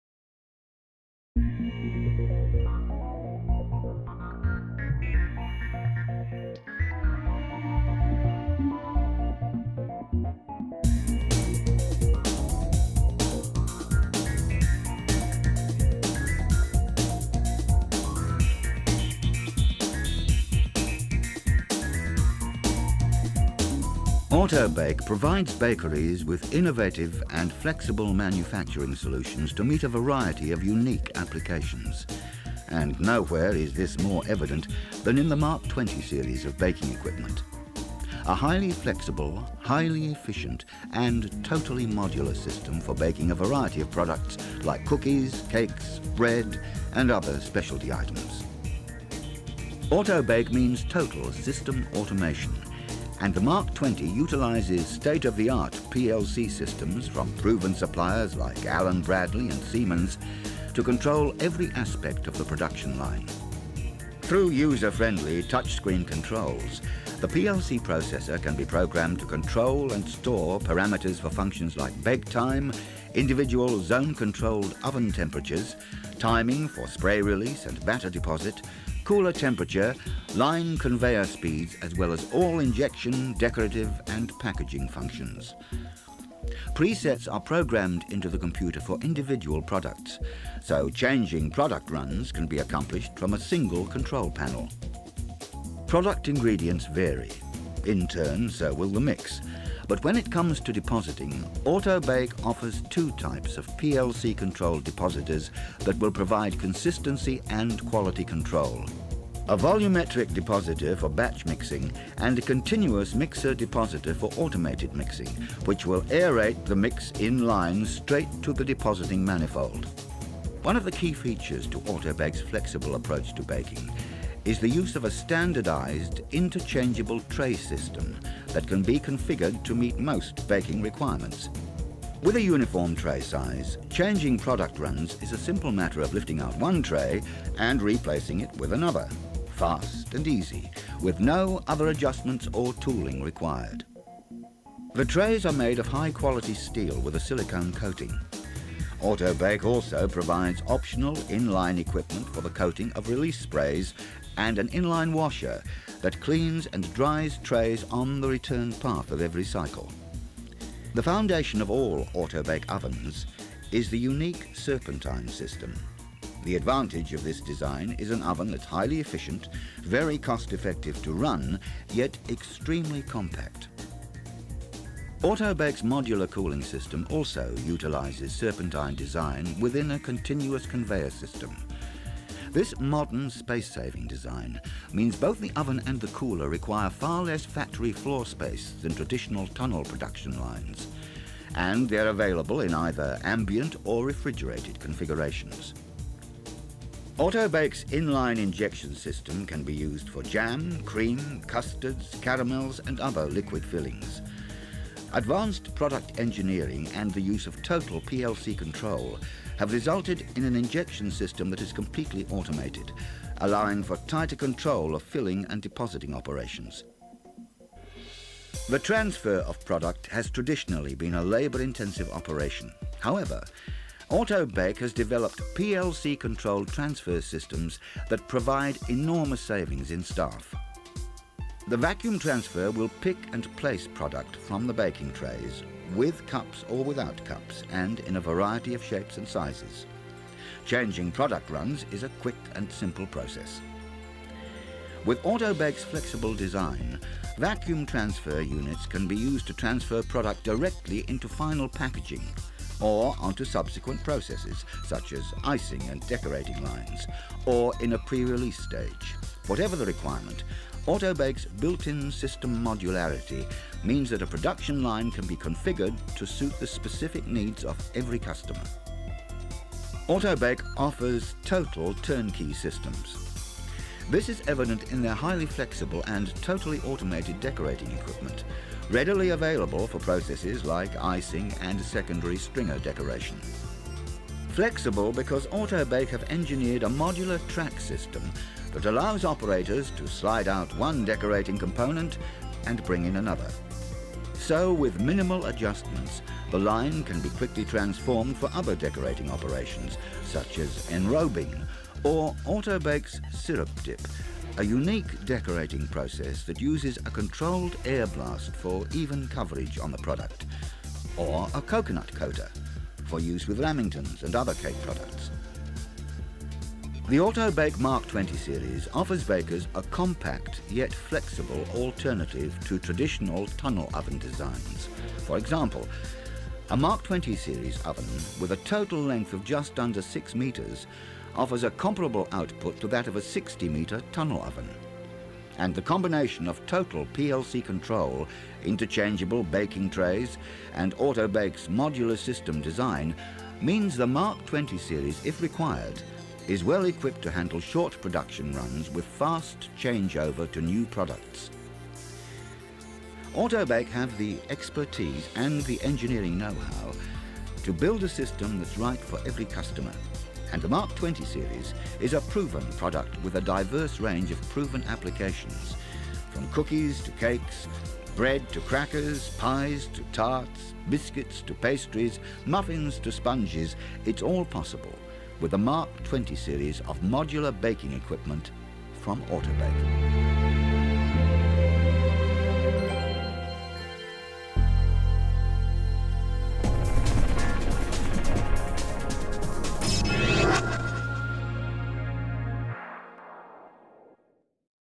AutoBake Core Technology
AUTO-BAKE offers an array of modular industrial baking solutions that can revolutionize your plant.  From in-feed to out-feed, no other system can deliver the flexibility and profitability of an Auto-Bake baking system.

The heart of Auto-Bake's technology is the ultra-compact Serpentine oven, where trays are conveyed across multiple horizontal levels in a distinctive S-shape, achieving a footprint one-tenth the size of tunnel ovens.  This space saving Serpentine technology has also been incorporated within the proofing and cooling modules, resulting in one of the most compact baking systems available today.  Enormous advantages are provided over conventional technologies.
UNPRECEDENTED CONTROL & AUTOMATION LEVELS for accurate temperature and line speed.
SERPENTINE COOLERS provide a controlled environment to ensure the highest quality finish.
AUTOMATIC DE-PANNING incorporates state-of-the-art robotics for Auto-Bake product transfer.